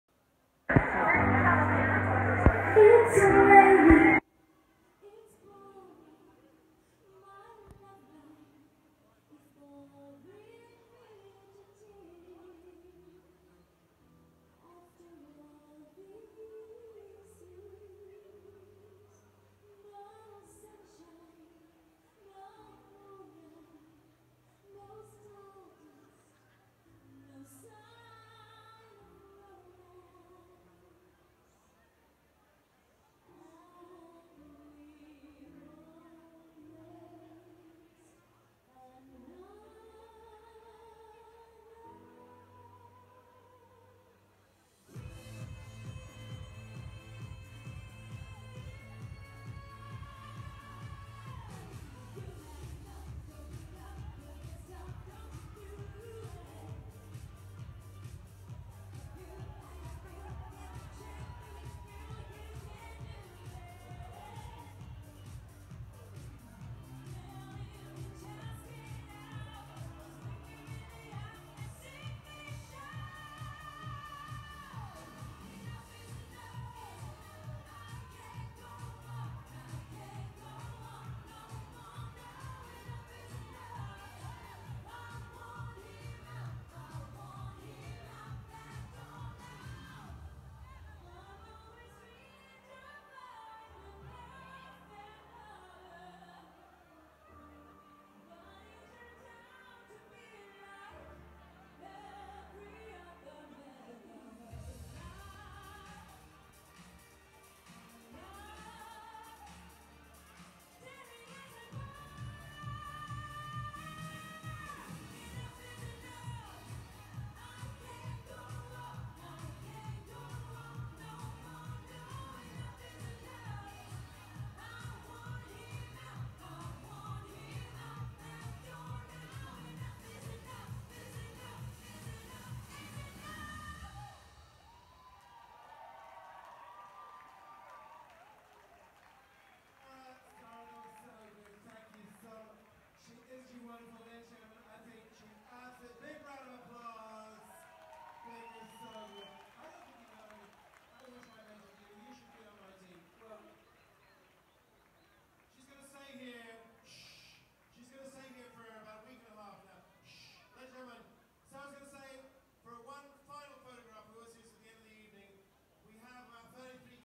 Saara Aalto singing at Product of the Year Awards 2017
Watch Saara Aalto’s brilliant performance at at Product of the Year awards 2017.